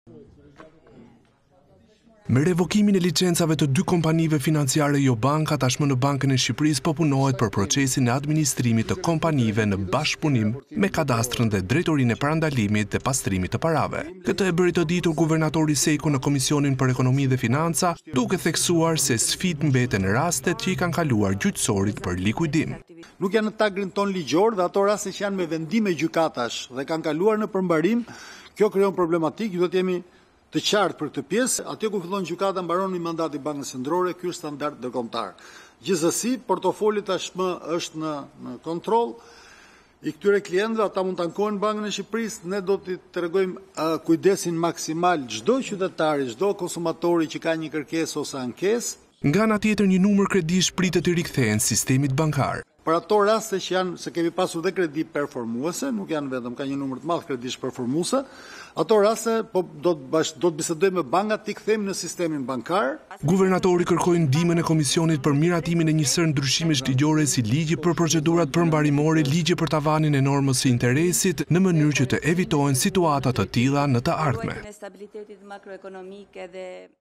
Ku fillon gjykata mbaron mandati i Bankës, Sejko: Ka plane për likujdimin e kompanive
Ku fillon gjykata mbaron mandati i bankës - Sejko: Kanë plan per likujdimin e kompanive që u është revokuar liçenca